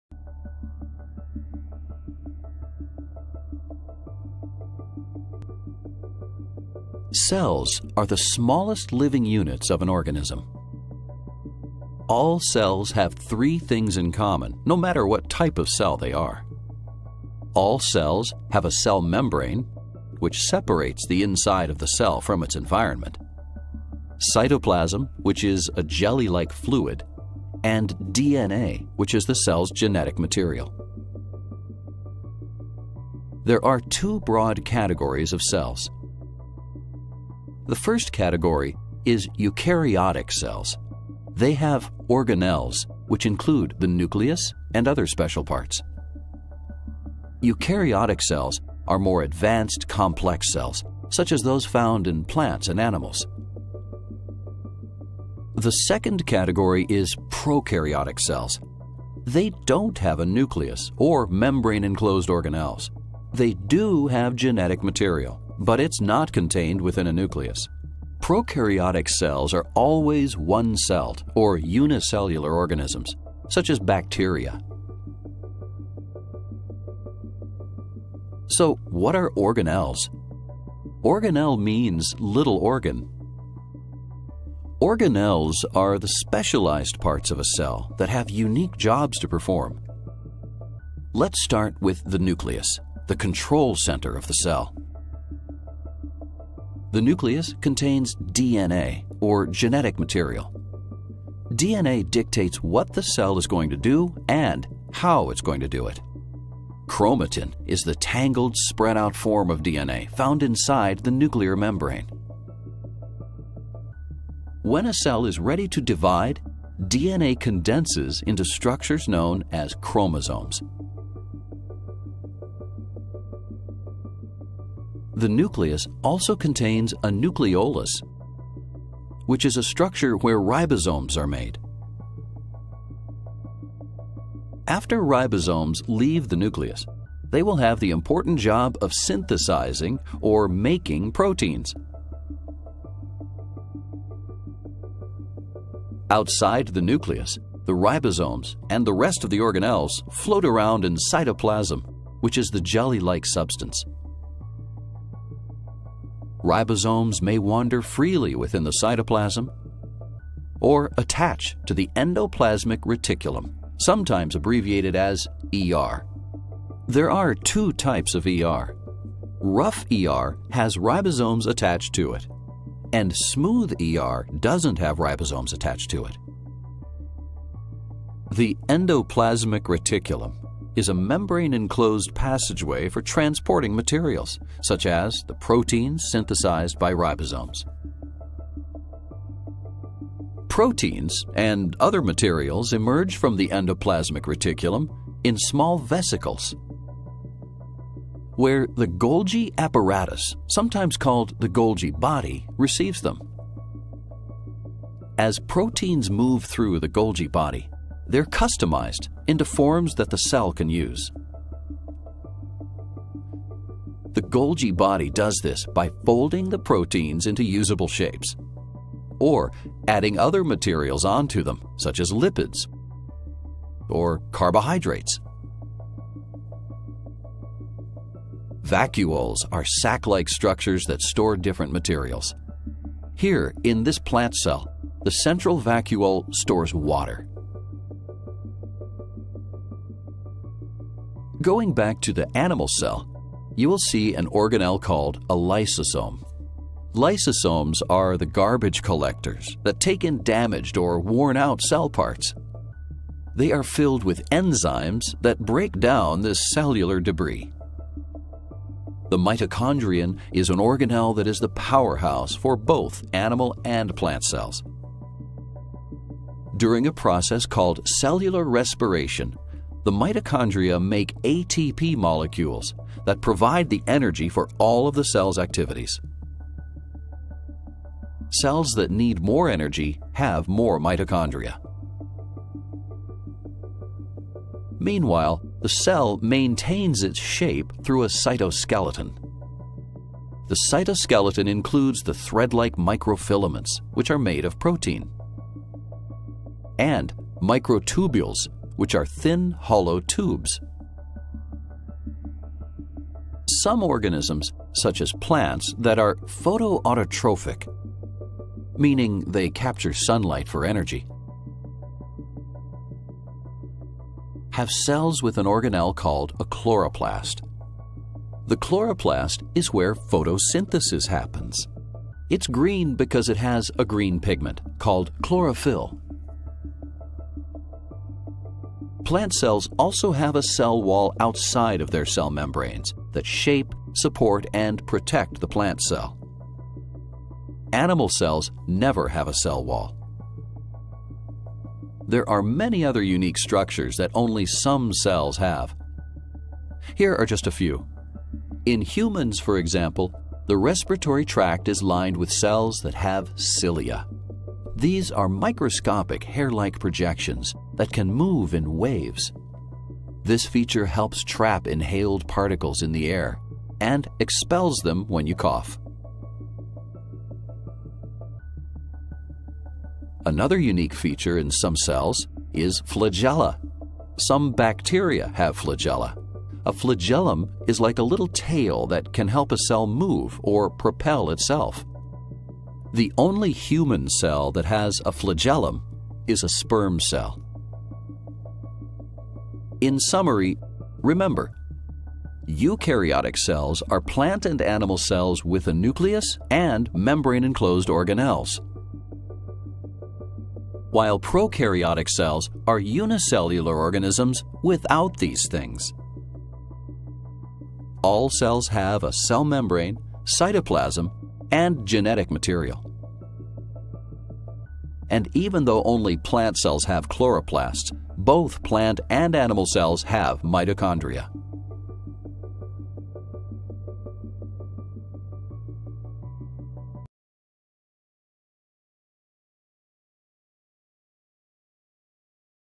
Cell structure & Function - An amazing HD and 3D Animated Video
Cell is the basic functional unit of life. This video visually takes you through the cell in HD format. The brilliant presentation was produced by Nucleus Medical Media.
I truly hope this video will enhance your understanding amplify your interest  in the sciences.